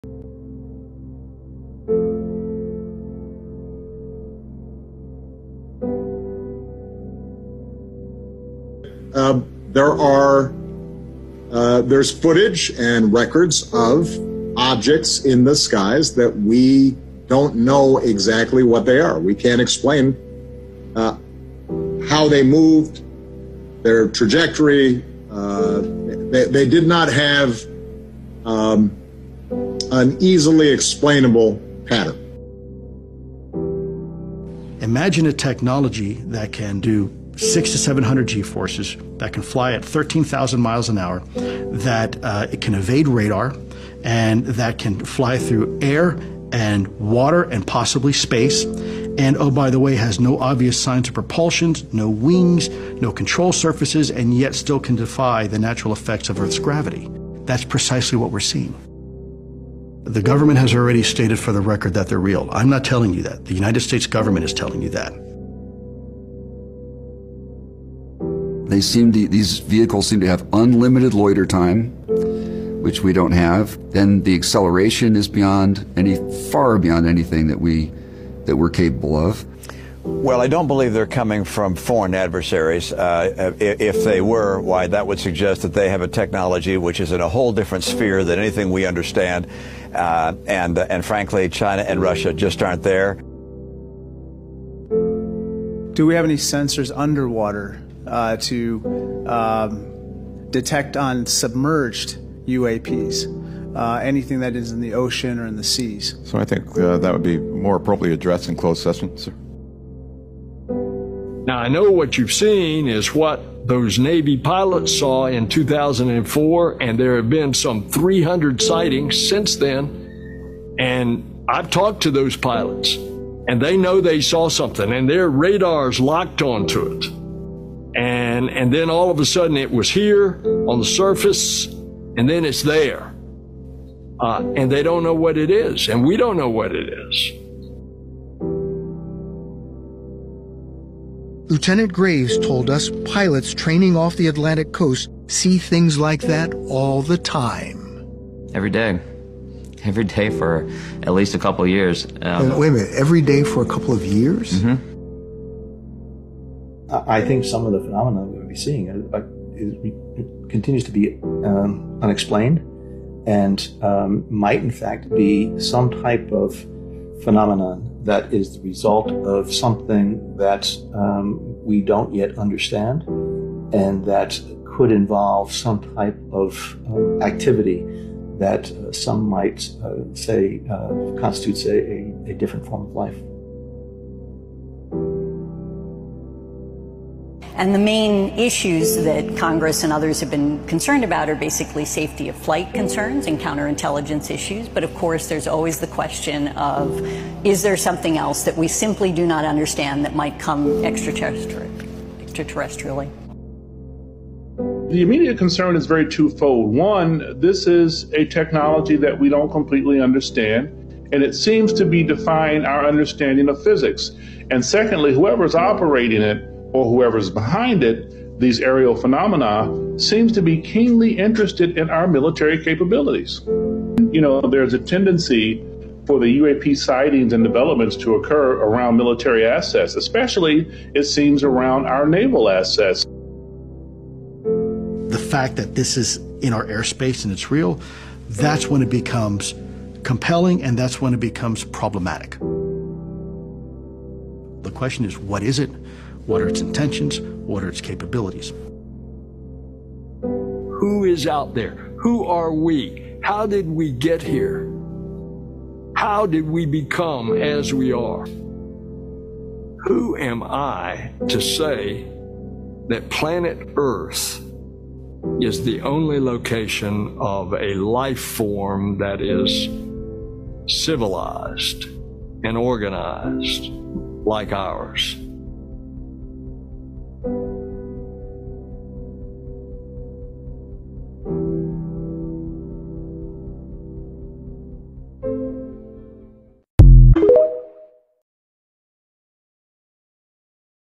UFOs Government Acknowlegement
Short 5 minute video about Government knowledge of UAPs.